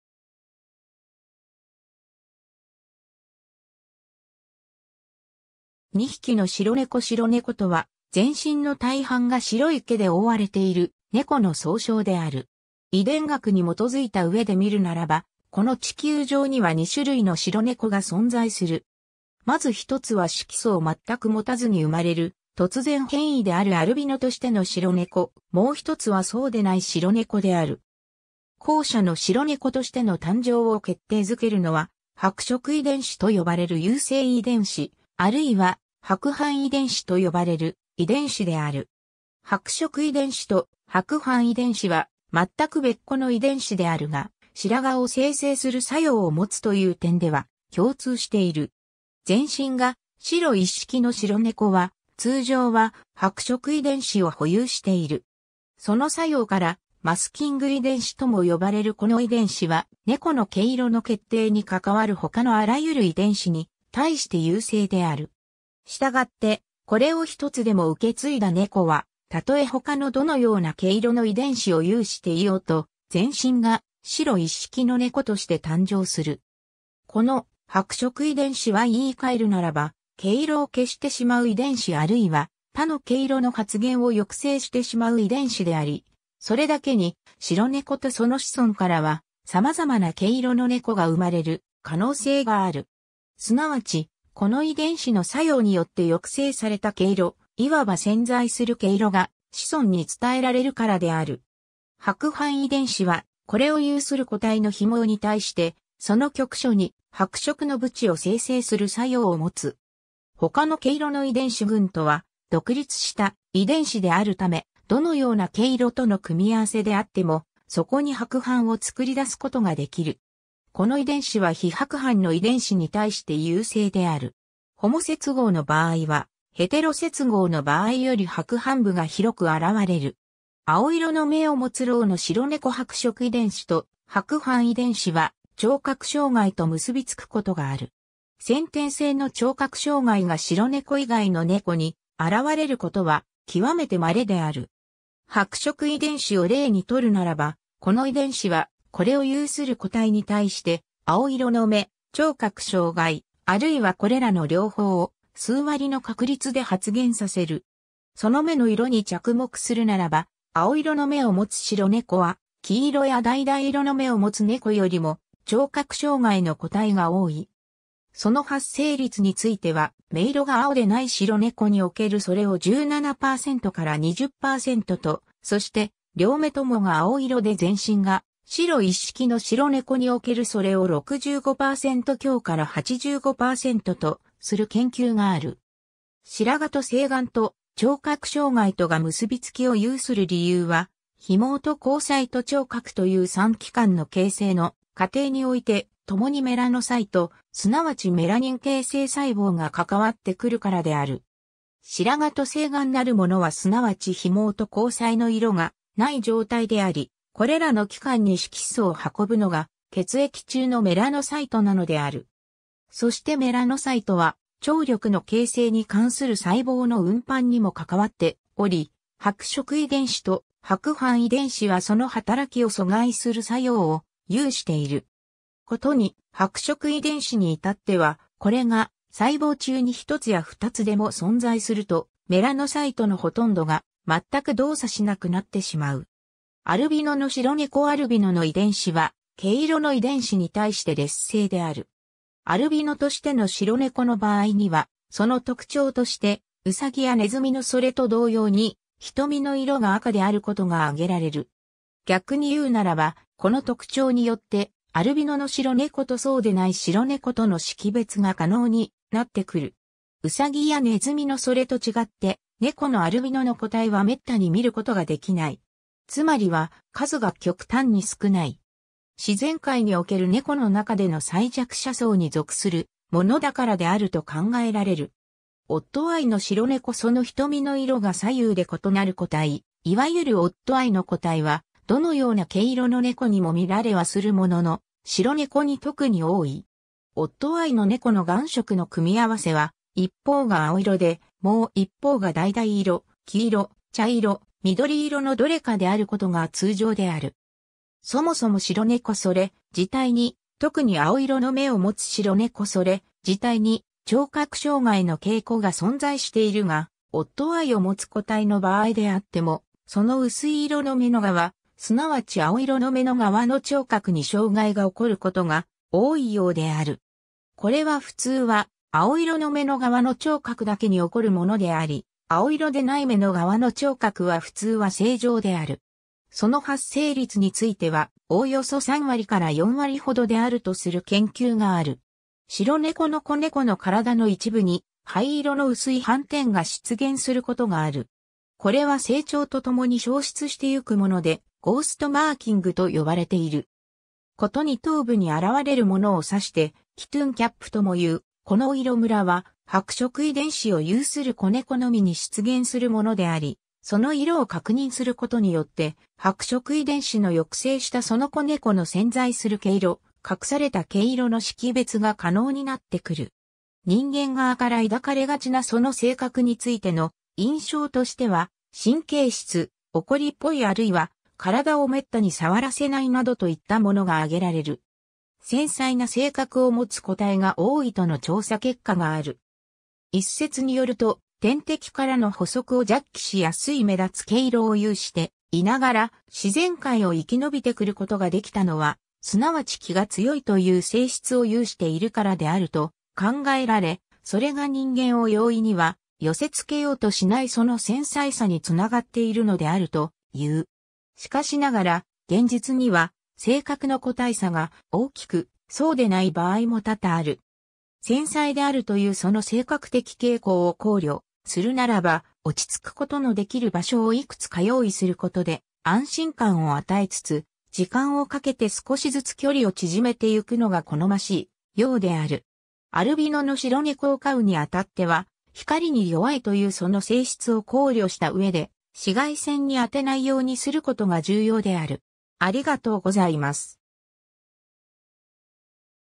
白猫
2匹の白猫 白猫（しろねこ）とは、全身の大半が白い毛で覆われている猫の総称である。
 遺伝学に基づいたうえで見るならば、この地球上には2種類の白猫が存在する。
まず1つは色素を全く持たずに生まれる、突然変異であるアルビノとしての白猫、もう1つはそうでない白猫である。
後者の白猫としての誕生を決定付けるのは、「白色遺伝子」と呼ばれる優性遺伝子、あるいは「白斑遺伝子」と呼ばれる遺伝子である。
白色遺伝子と白斑遺伝子は全く別個の遺伝子であるが、白毛を生成する作用を持つという点では共通している。
 全身が白一色の白猫は、通常は白色遺伝子を保有している。
 その作用から「マスキング遺伝子」(遮蔽遺伝子)とも呼ばれるこの遺伝子は、猫の毛色の決定に関わる他のあらゆる遺伝子に対して優性である。
したがって、これを1つでも受け継いだ猫は、たとえ他のどのような毛色の遺伝子を有していようと、全身が白一色の猫として誕生する。
 この「白色遺伝子」は言い換えるならば「毛色を消してしまう遺伝子」あるいは「他の毛色の発現を抑制してしまう遺伝子」であり、それだけに、白猫とその子孫からは様々な毛色の猫が生まれる可能性がある。
 すなわち、この遺伝子の作用によって抑制された毛色―いわば潜在する毛色が子孫に伝えられるからである。
 白斑遺伝子は、これを有する個体の被毛に対して、その局所に白色の斑を生成する作用を持つ。
 他の毛色の遺伝子群とは独立した遺伝子であるため、どのような毛色との組み合わせであっても、そこに白斑を作り出すことができる。
この遺伝子は非白斑の遺伝子に対して優性である。
ホモ接合の場合はヘテロ接合の場合より白斑部が広く現われる。
 青色の眼を持つ聾の白猫 白色遺伝子と白斑遺伝子は聴覚障害と結び付くことがある。
 先天性の聴覚障害が白猫以外の猫に現われることは極めて稀である。
白色遺伝子を例に取るならば、この遺伝子は、これを有する個体に対して、青色の眼、聴覚障害、あるいはこれらの両方を、数割の確率で発現させる。
その眼の色に着目するならば、青色の眼を持つ白猫は、黄色や橙色の眼を持つ猫よりも聴覚障害の個体が多い。
その発生率については、目色が青でない白猫におけるそれを17%から20%と、そして両目ともが青色で全身が白一色の白猫におけるそれを65%強から85%とする研究がある。
 白毛と青眼と聴覚障害とが結び付きを有する理由は、被毛と虹彩と聴覚という3器官の形成の過程において、ともにメラノサイト―すなわちメラニン形成細胞が関わってくるからである。
白毛と青眼なるものはすなわち被毛と虹彩の色が無い状態であり、これらの器官に色素を運ぶのが血液中のメラノサイトなのである。
そしてメラノサイトは聴力の形成に関する細胞の運搬にも関わっており、白色遺伝子と白斑遺伝子はその働きを阻害する作用を有している。
 ことに白色遺伝子に至っては、これが細胞中に1つや2つでも存在すると、メラノサイトのほとんどが全く動作しなくなってしまう。
 アルビノの白猫 アルビノの遺伝子は毛色の遺伝子に対して劣性である。
アルビノとしての白猫の場合には、その特徴として、ウサギやネズミのそれと同様に、瞳の色が赤であることが挙げられる。
逆に言うならば、この特徴によって、アルビノの白猫とそうでない白猫との識別が可能になってくる。
 ウサギやネズミのそれと違って、猫のアルビノの個体は滅多に見ることができない。
つまりは数が極端に少ない。
自然界における猫の中での最弱者層に属するものだからであると考えられる。
 オッドアイの白猫 その瞳の色が左右で異なる個体―いわゆるオッドアイの個体は、どのような毛色の猫にも見られはするものの、白猫に特に多い。
オッドアイの猫の眼色の組み合わせは、一方が青色で、もう一方が橙色/黄色/茶色/緑色のどれかであることが通常である。
 そもそも白猫それ自体に―特に青色の眼を持つ白猫それ自体に聴覚障害の傾向が存在しているが、オッドアイを持つ個体の場合であっても、その薄い色の眼の側―すなわち青色の眼の側の聴覚に障害が起こることが多いようである。
 これは普通は青色の眼の側の聴覚だけに起こるものであり、青色でない眼の側の聴覚は普通は正常である。
その発生率については、おおよそ3割から4割ほどであるとする研究がある。
 白猫の子猫の身体の一部に灰色の薄い斑点が出現することがある。
これは成長とともに消失してゆくもので、「ゴースト・マーキング」と呼ばれている。
 ことに頭部に現われるものを指して「キトゥン・キャップ」ともいうこ...